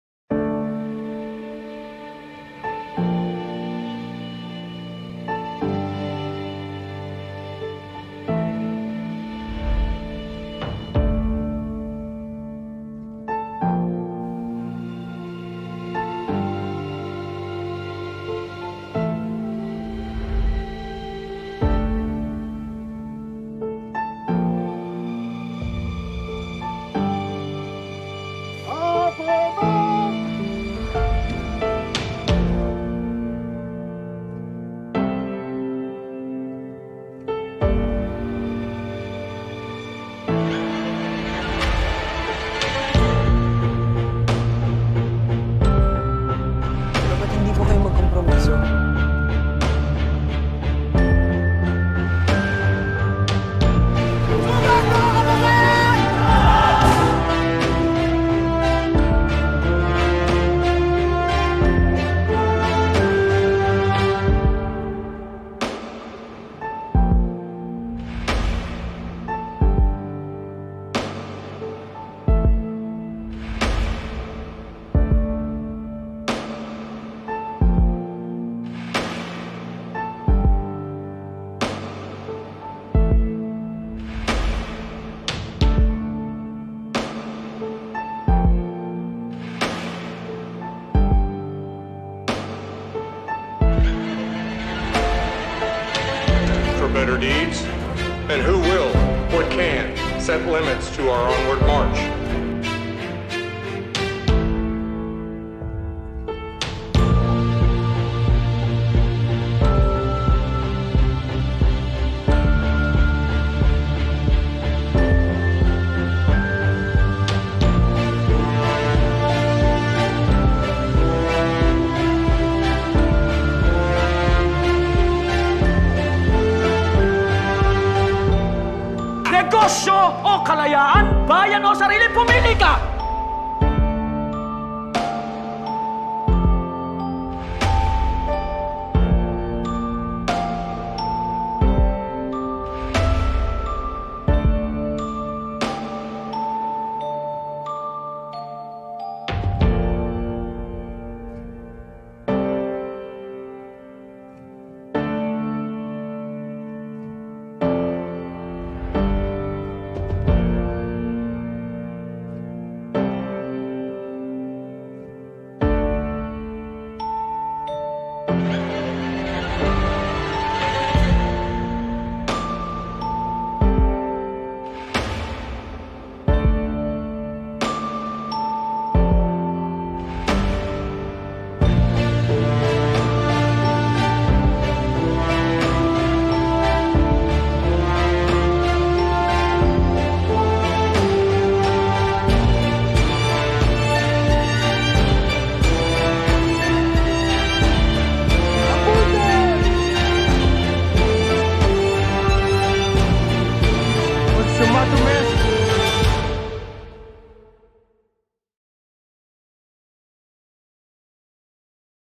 Artist Reception: "The Hero in Our Time" by Juanito Torres
Our current society continues to be faced with ever-developing and newer threats. There is the continuing threat of the pandemic, climate change, economic difficulties brought about by the world conflict, increasing authoritarianism all over the world, and the aggressive behavior of countries with a disproportionate amount of power and military might. Torres’ exhibit leads his public to the inevitable conclusion that we are indeed the heroes of our time.
As Dalisay astutely observes, “Even as we see St. Michael battling the dragon, we are St. Michael.” The only thing is “who and what is the dragon of our time?” But just like our heroes of yesteryear, the Filipinos of today shall prevail.
The exhibit ran from August 27 to September 7, 2022.
Galerie Raphael U.P. Town Center
2/F Phase 2, U.P. Town Center
Katipunan Ave., Diliman, Quezon City